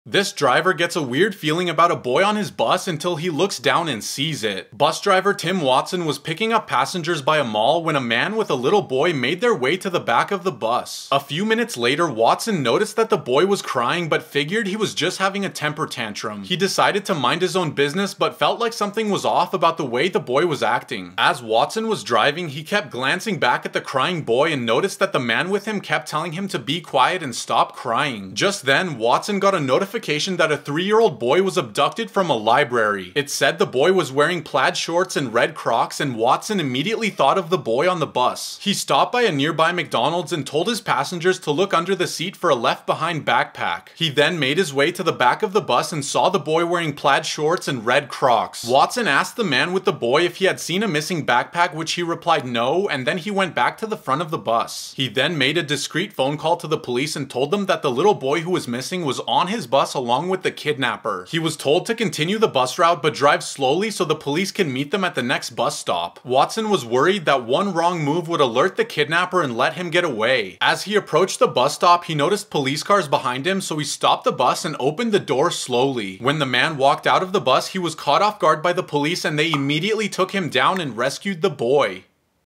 Driver Gets A WEIRD Feeling About A Boy On His Bus...
The bus driver has amazing instincts!